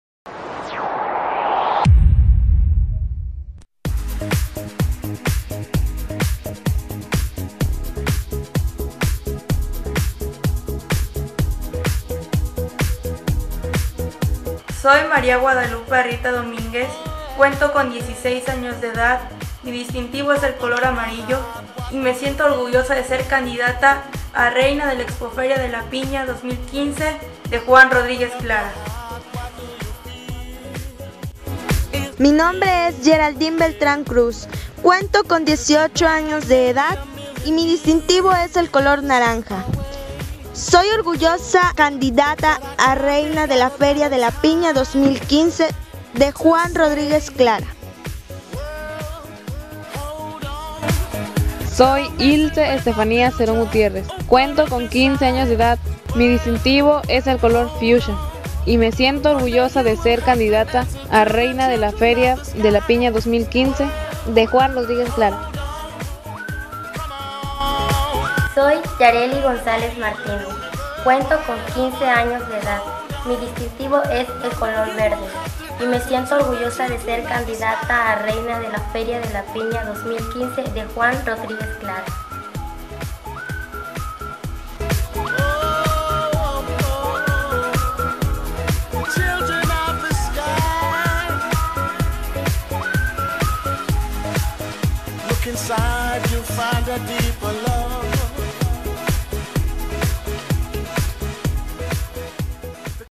Candidatas a Reina de la Piña 2015
Hermosas candidatas a Reina de la Feria de la Piña 2015 de Juan Rodríguez Clara. Guadalupe, distintivo amarillo; Yeraldín, distintivo naranja; Estefanía distintivo Rosa y Yarely distintivo verde